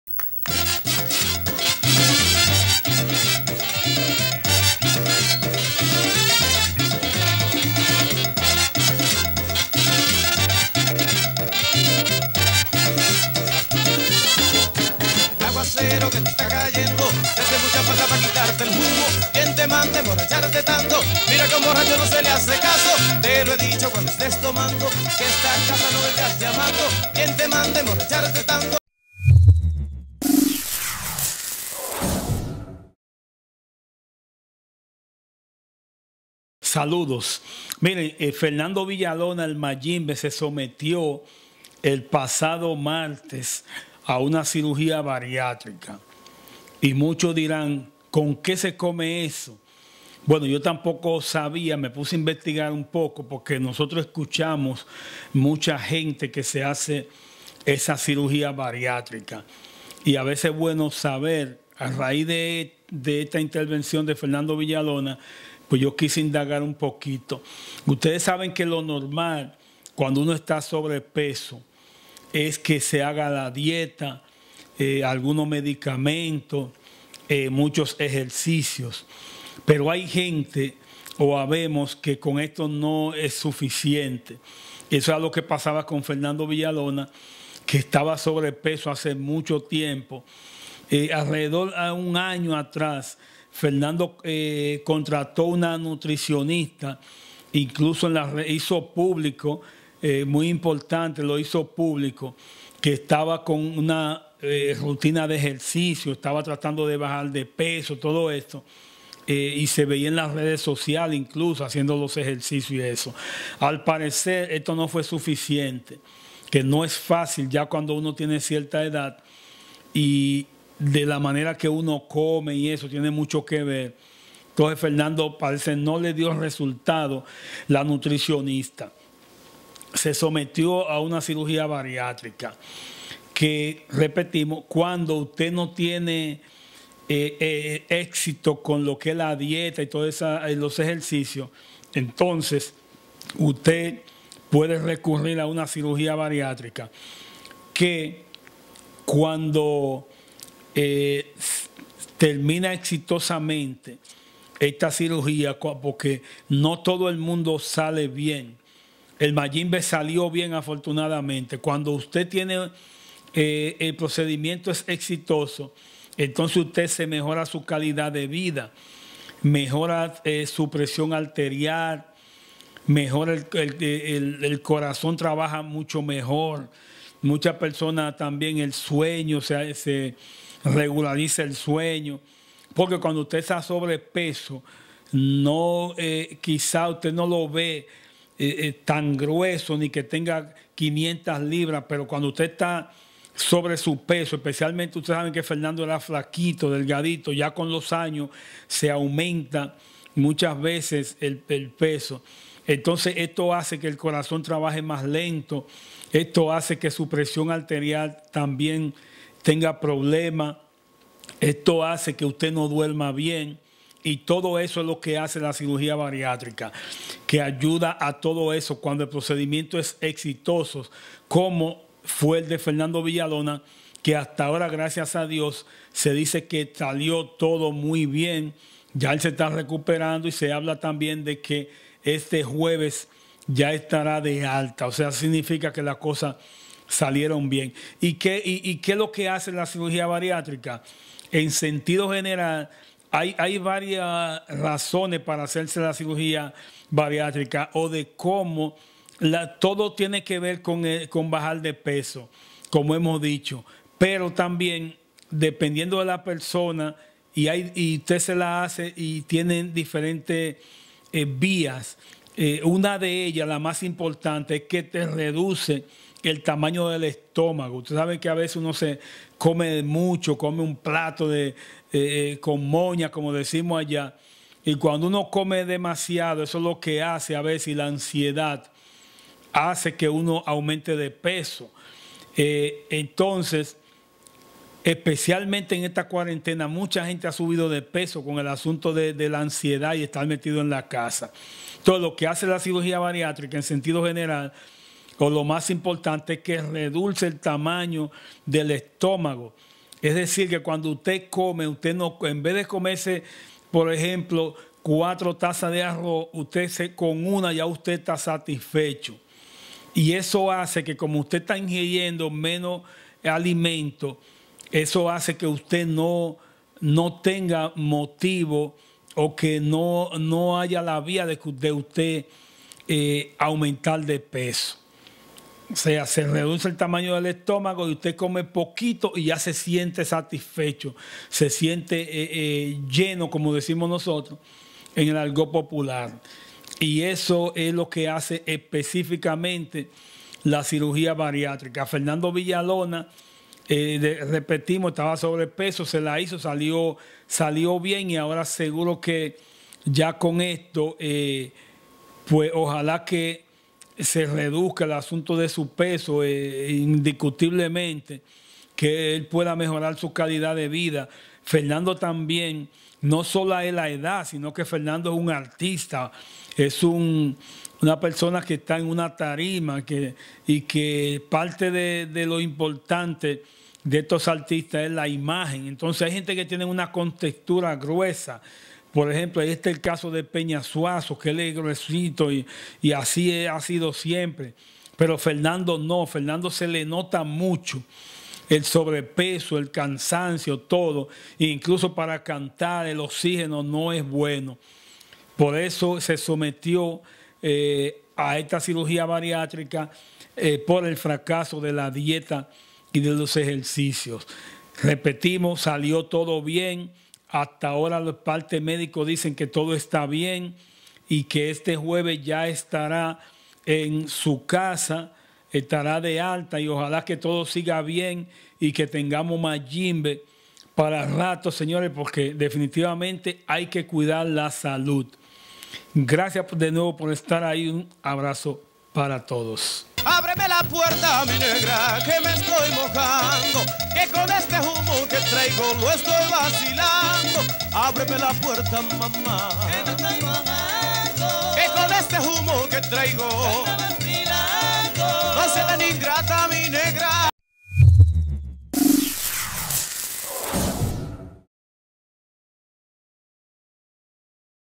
el mayimbe '' Fernando Villalona '' se somete a cirugia bariatrica, te contamos como salio todo...
este es un canal de entretenimiento e informacion para adultos...